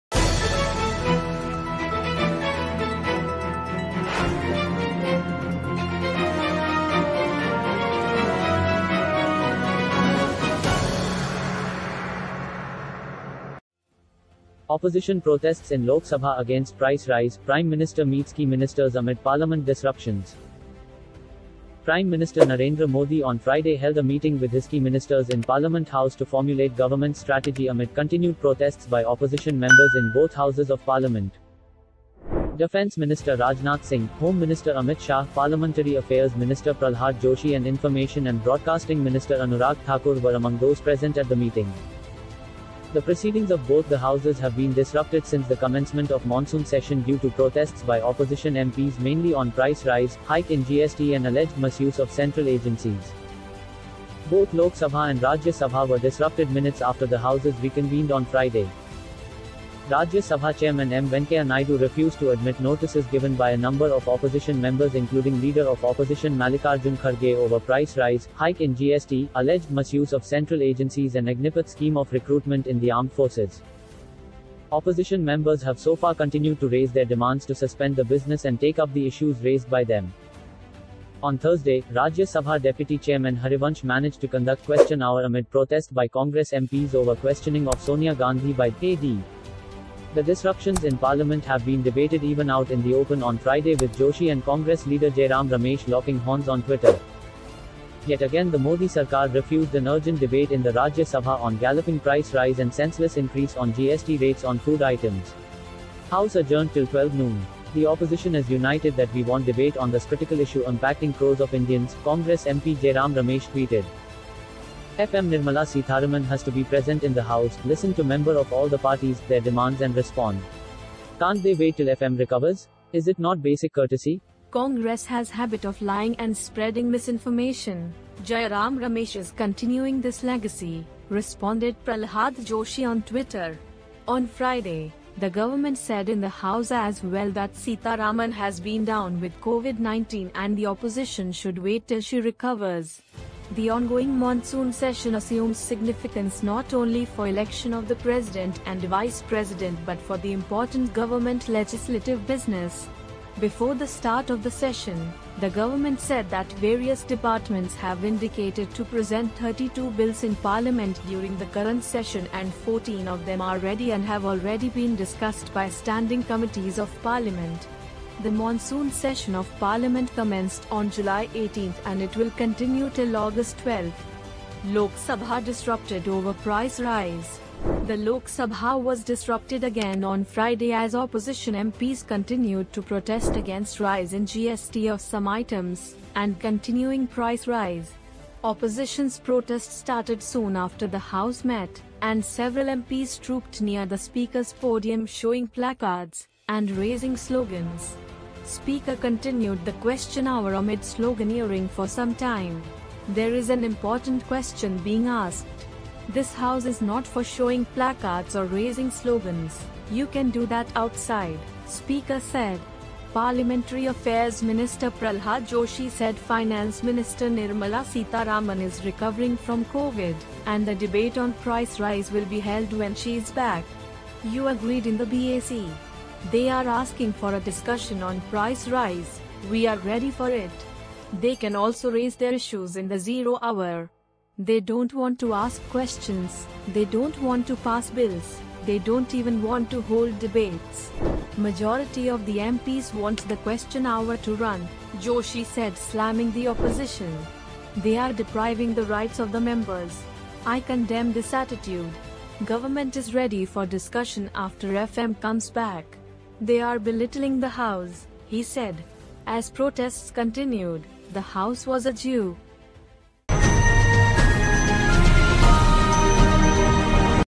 Opposition protests Lok Sabha against price rise PM meets key ministers Parliament disruptions
Prime Minister Narendra Modi on Friday held a meeting with his key ministers in Parliament House to formulate government's strategy amid continued protests by Opposition members in both Houses of Parliament.

Defence Minister Rajnath Singh, Home Minister Amit Shah, Parliamentary Affairs Minister Pralhad Joshi and Information and Broadcasting Minister Anurag Thakur were among those present at the meeting.

The proceedings of both the Houses have been disrupted since the commencement of Monsoon Session due to protests by Opposition MPs mainly on price rise, hike in GST and alleged misuse of central agencies.

Both Lok Sabha and Rajya Sabha were disrupted minutes after the Houses reconvened on Friday.

Rajya Sabha Chairman M. Venkaiah Naidu refused to admit notices given by a number of Opposition members including Leader of Opposition Mallikarjun Kharge over price rise, hike in GST, alleged misuse of central agencies and Agnipath scheme of recruitment in the armed forces.

Opposition members have so far continued to raise their demands to suspend the business and take up the issues raised by them.

On Thursday, Rajya Sabha Deputy Chairman Harivansh managed to conduct Question Hour amid protest by Congress MPs over questioning of Sonia Gandhi by the ED.

The disruptions in Parliament have been debated even out in the open on Friday with Joshi and Congress leader Jairam Ramesh locking horns on Twitter.

"Yet again the Modi Sarkar refused an urgent debate in the Rajya Sabha on galloping price rise and senseless increase on GST rates on food items. House adjourned till 12 noon. The Opposition is united that we want debate on this critical issue impacting crores of Indians," Congress MP Jairam Ramesh tweeted.

"FM Nirmala Sitharaman has to be present in the house, listen to member of all the parties, their demands and respond. Can't they wait till FM recovers? Is it not basic courtesy? Congress has habit of lying and spreading misinformation, Jairam Ramesh is continuing this legacy," responded Pralhad Joshi on Twitter.

On Friday, the government said in the House as well that Sitharaman has been down with Covid 19 and the Opposition should wait till she recovers.

The ongoing Monsoon Session assumes significance not only for election of the President and Vice President but for the important government legislative business.

Before the start of the Session, the government said that various departments have indicated to present 32 bills in Parliament during the current session and 14 of them are ready and have already been discussed by Standing Committees of Parliament.

The Monsoon Session of Parliament commenced on July 18 and it will continue till August 12.

Lok Sabha disrupted over price rise

The Lok Sabha was disrupted again on Friday as Opposition MPs continued to protest against rise in GST of some items, and continuing price rise.

Opposition's protest started soon after the House met, and several MPs trooped near the Speaker's podium showing placards, and raising slogans.

Speaker continued the Question Hour amid sloganeering for some time.

"There is an important question being asked... This House is not for showing placards or raising slogans, you can do that outside," Speaker said.

Parliamentary Affairs Minister Pralhad Joshi said Finance Minister Nirmala Sitharaman is recovering from Covid, and the debate on price rise will be held when she is back.

"You agreed in the BAC... They are asking for a discussion on price rise, we are ready for it. They can also raise their issues in the Zero Hour.

"They don't want to ask questions, they don't want to pass bills, they don't even want to hold debates... Majority of the MPs want the Question Hour to run," Joshi said slamming the Opposition.

"They are depriving the rights of the members. I condemn this attitude. Government is ready for discussion after FM comes back... They are belittling the House," he said.

As protests continued, the House was adjou